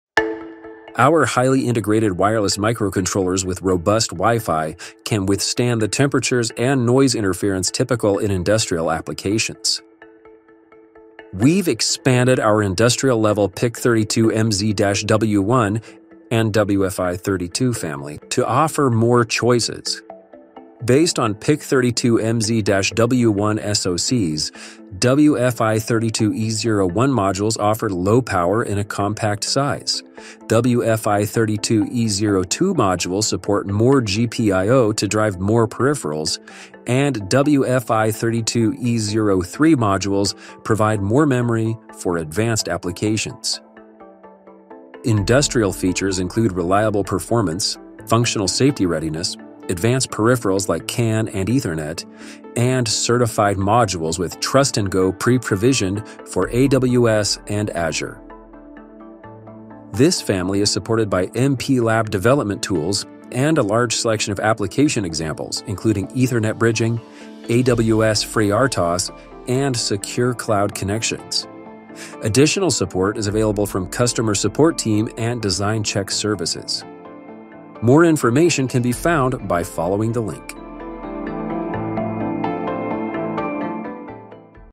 PIC32MZ-W1 and WFI32 Wi-Fi® MCU Family
Designed to thrive in harsh conditions and extreme environments, our PIC32MZ-W1 family of Wi-Fi® MCU modules and SoCs provides industrial-level features, robust connectivity and advanced security. The family includes a variety of options for memory and GPIOs so there are no limitations to your application.

With their tolerance to noise interference and extreme temperatures, these devices are well suited for industrial applications. Along with robust performance, our Wi-Fi MCUS are complemented by a simple development environment and turnkey software examples that make it easy for you to start your project.